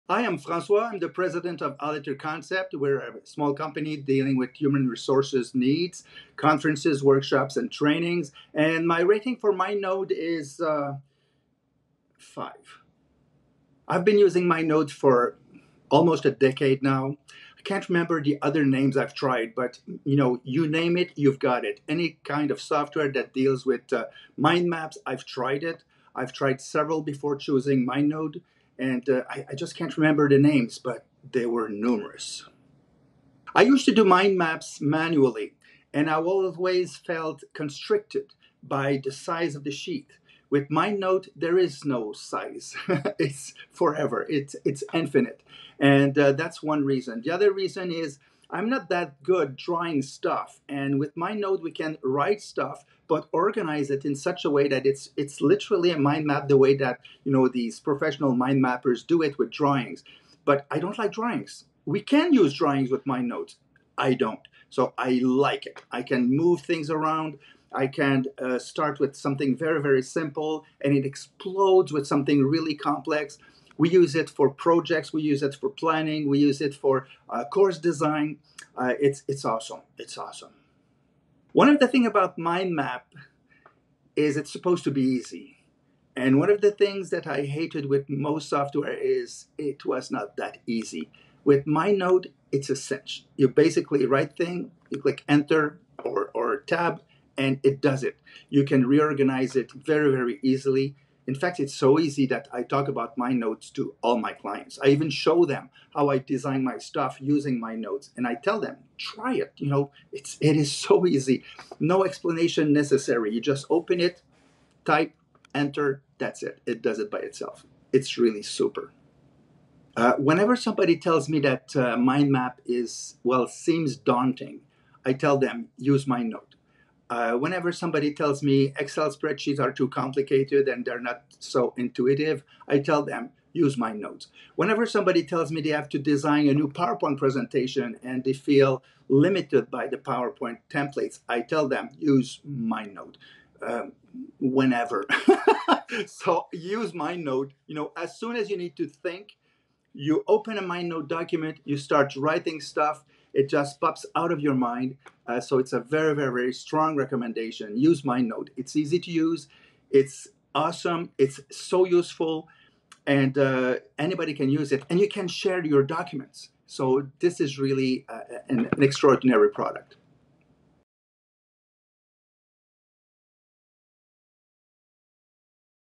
MindNode Review: MindNode Best in Class
"MindNode Review 2023.

0:00 Intro
0:13 What other software products did you consider or switch from?
0:33 What were the reasons you chose MindNode?
1:21 How easy was it to onboard and integrate MindNode into your business?
1:59 What recommendations do you have for others considering MindNode?"